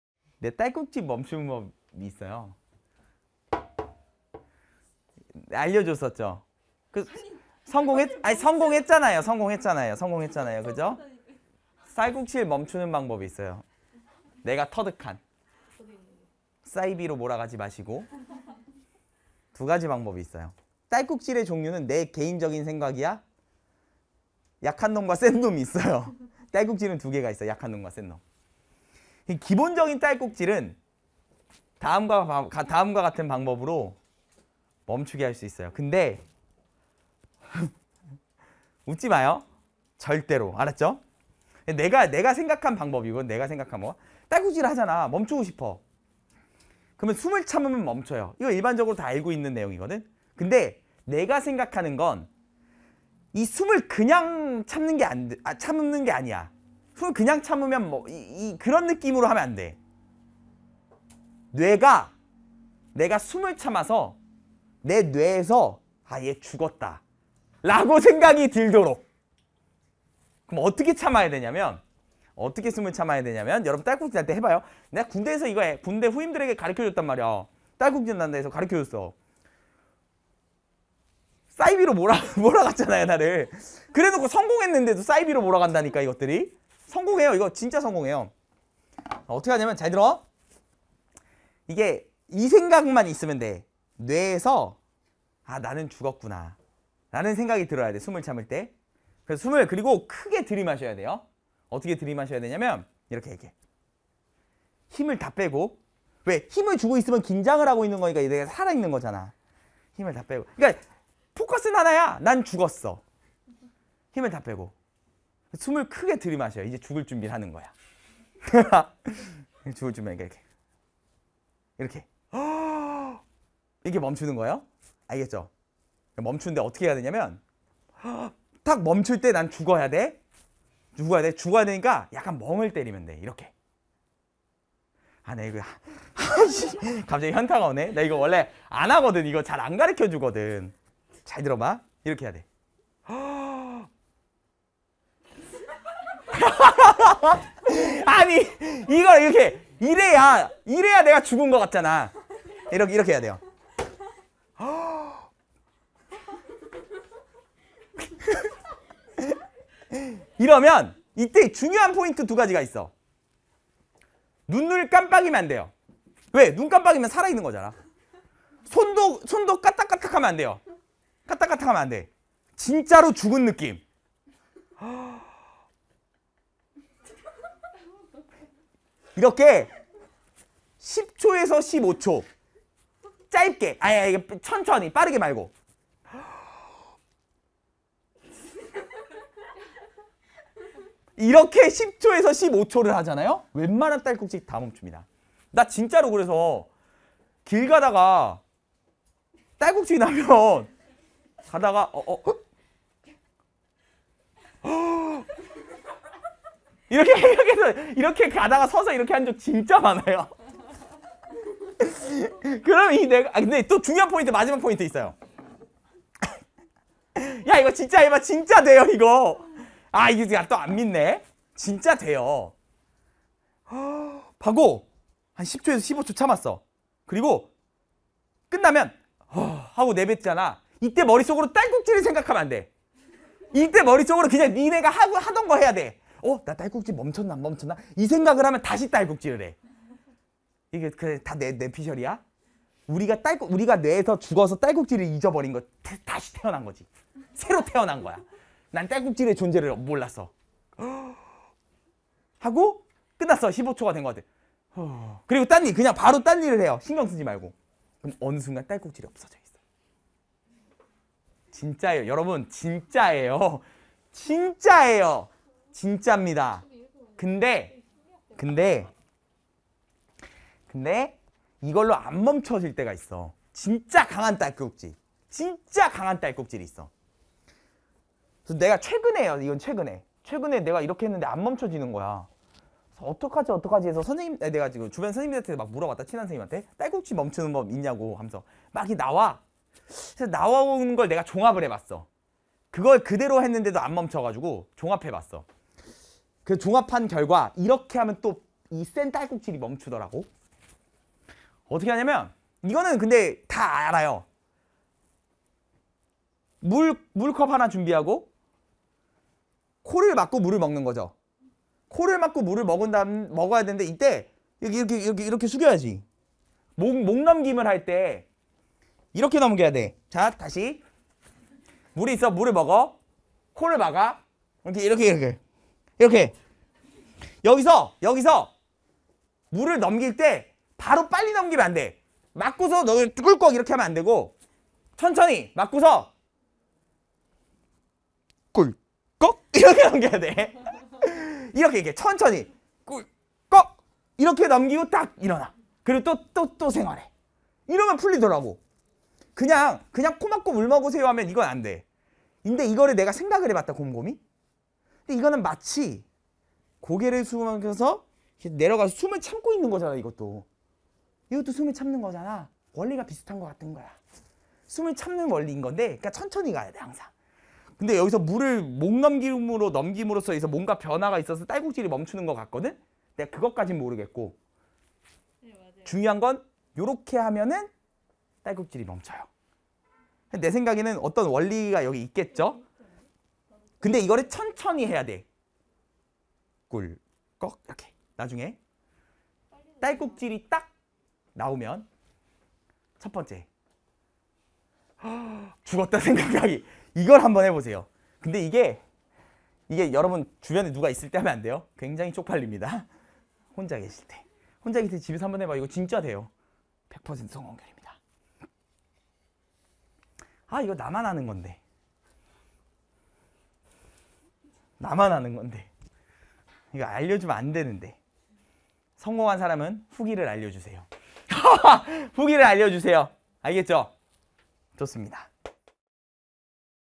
[메가스터디] 수학 하정민쌤 - 딸꾹질을 멈추는 방법?
수학 하정민쌤의 썰강!
딸꾹질을 멈추는 쌤만의 방법이 있다는데...